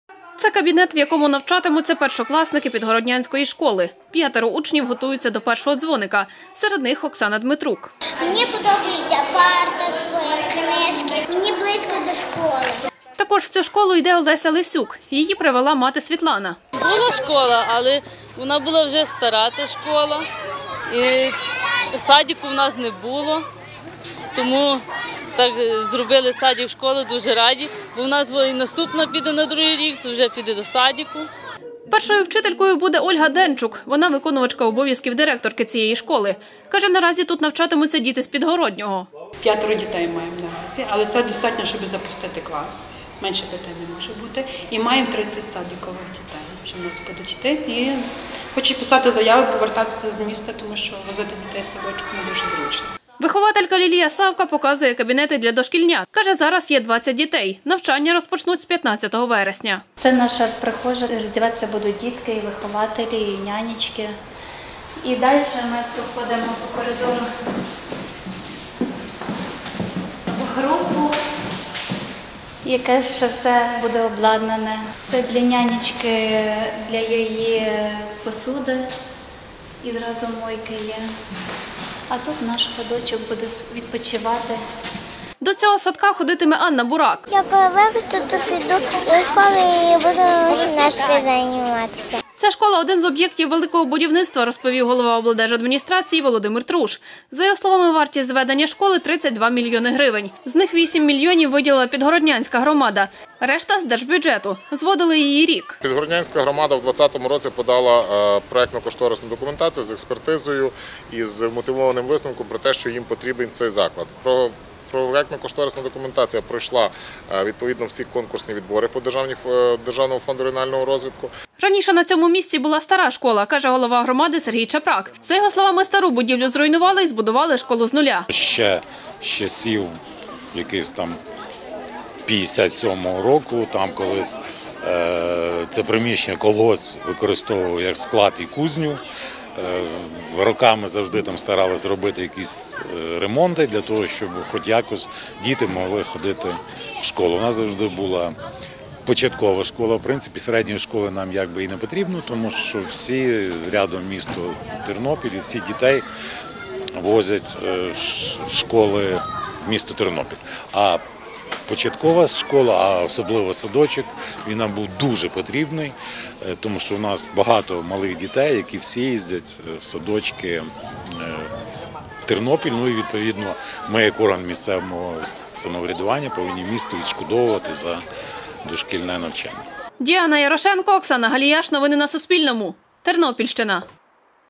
На Тернопільщині відкрили початкову школу з дитячим садком
Нову початкову школу з дитячим садком відкрили в Підгородньому. Ця школа розрахована на 70 дітей, розповів голова облдержадміністрації Володимир Труш